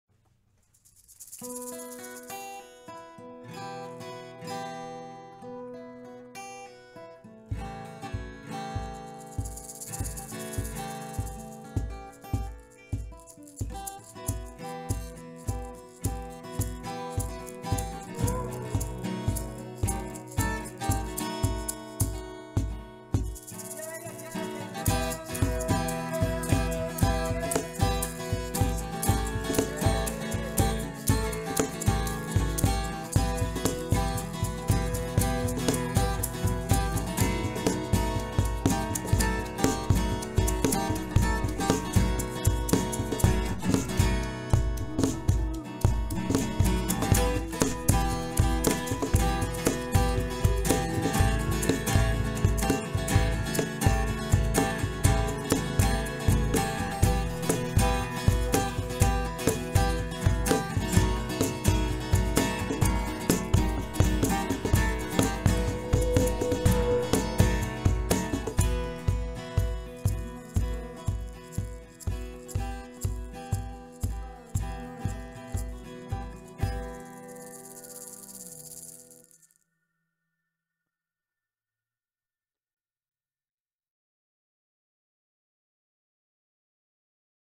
'Music Video' edited in Lightworks
A video I made to learn to use Lightworks (free movie editing software).